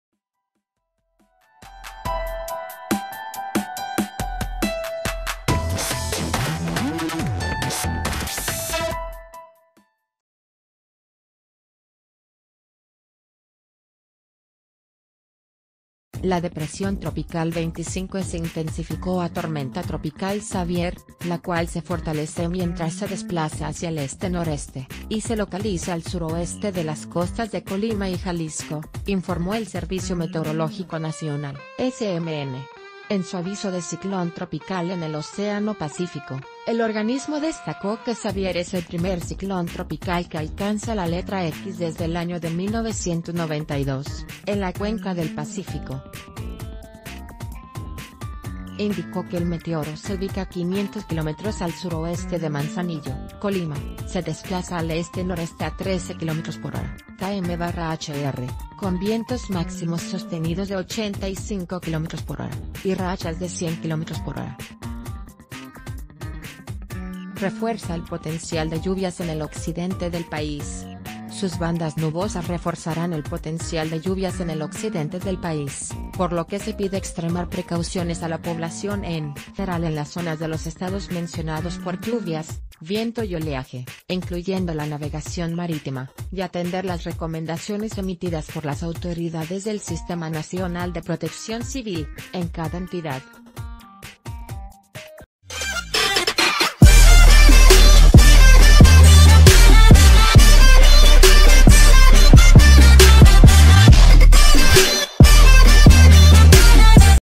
Tormenta tropical Xavier se fortalece en costas de Colima y Jalisco
La depresión tropical 25-E se intensificó a tormenta tropical Xavier, la cual se fortalecé mientras se desplaza hacia el este-noreste y se localiza al suroeste de las costas de Colima y Jalisco, informó el Servicio Meteorológico Nacional (SMN).En su aviso de Ciclón Tropical en el Océano Pacífico, el...